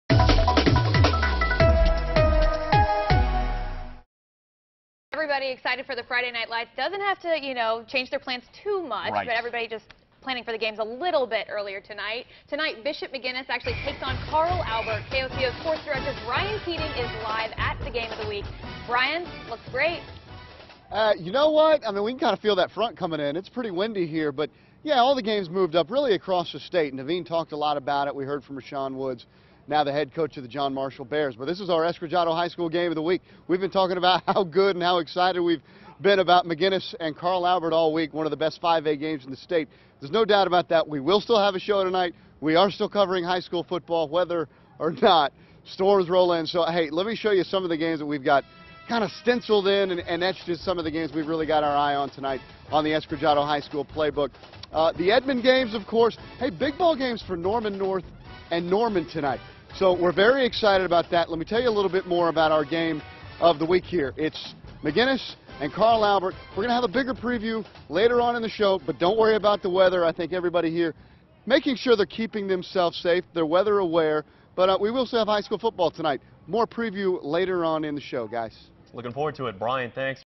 Week 5 game of the week
We're at Bishop McGuinnes vs. Carl Albert for our high school game of the week.